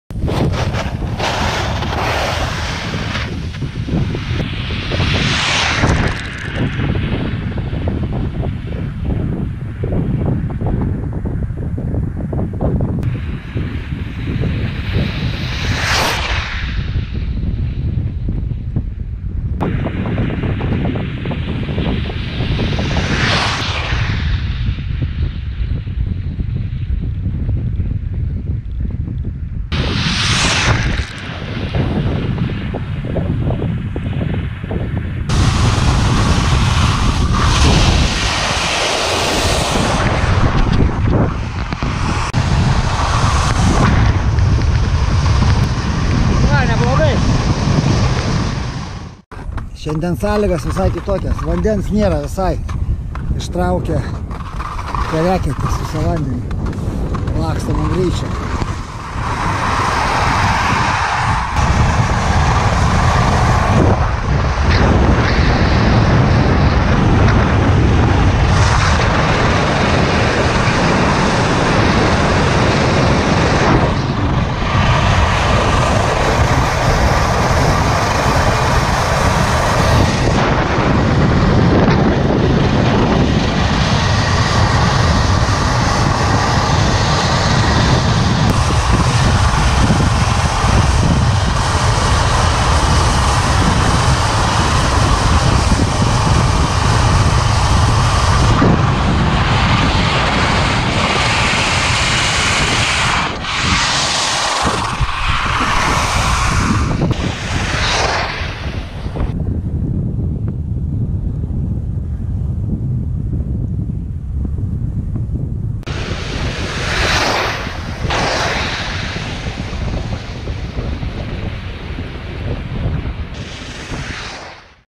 Jegos aitvarai kursai pamokos mokykla
Jegos aitvarai Naish, kursai mokymas 2021 Rekyva iceski kiteboarding
Max speed 82,5km/h
Wind 6-12m/s